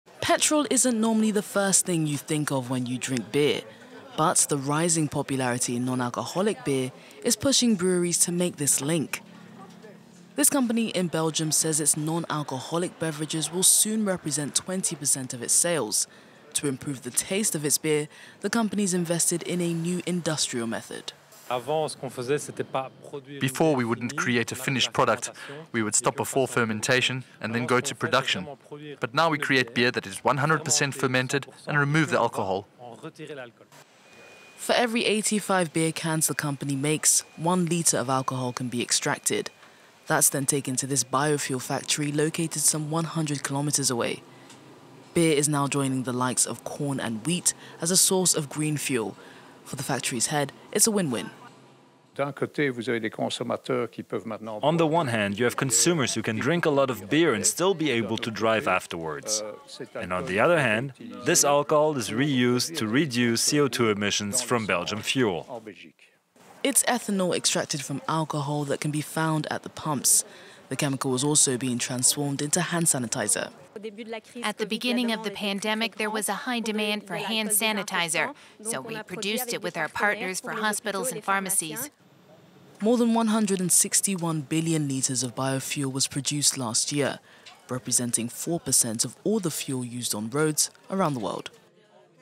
Alcohol extracted during beer production turned into biofuel • FRANCE 24 English
Thanks to a tie-up between brewing giant AB InBev and Brussels-based biofuels firm Alcogroup, alcohol removed in the process of creating alcohol-free beer is now being used to make biofuel.